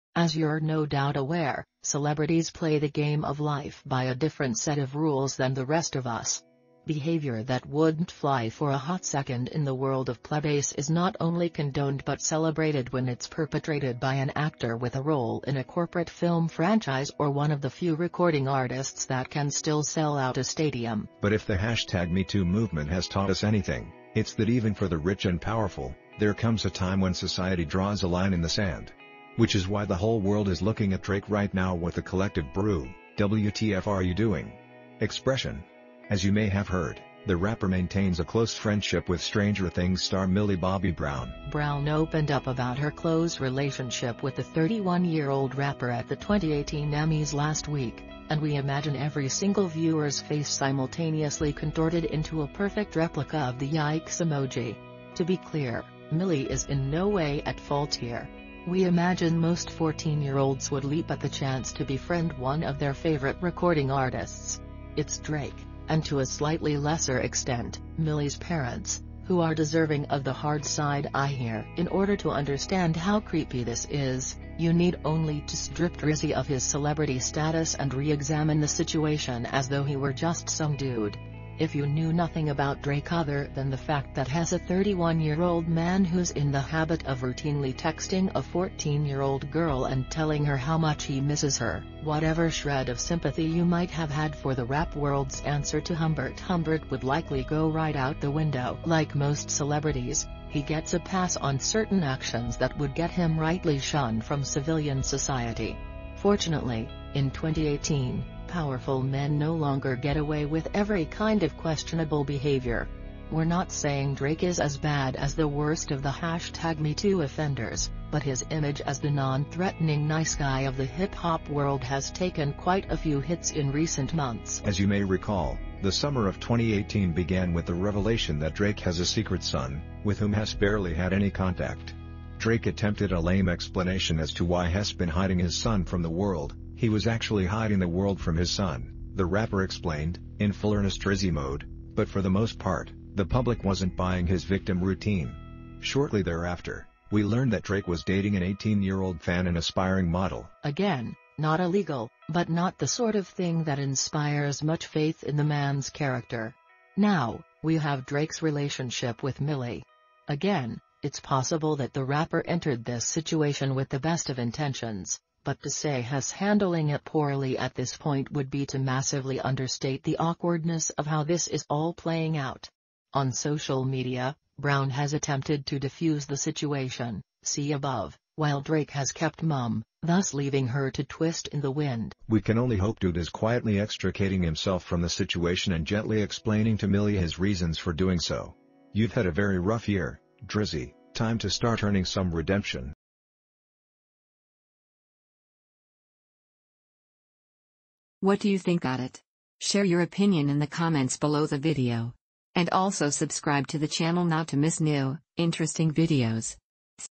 DRAKES FRIENDSHIP WITH MILLIE BOBBY BROWN IS CREEPY AF
As youre no doubt aware, celebrities play the game of life by a different set of rules than the rest of us.  Behavior that wouldnt fly for a hot second in the world of plebes is not only condoned but celebrated when its perpetrated by an actor with a role in a corporate film franchise or one of the few recording artists that can still sell out a stadium.
/celebrity-gossip/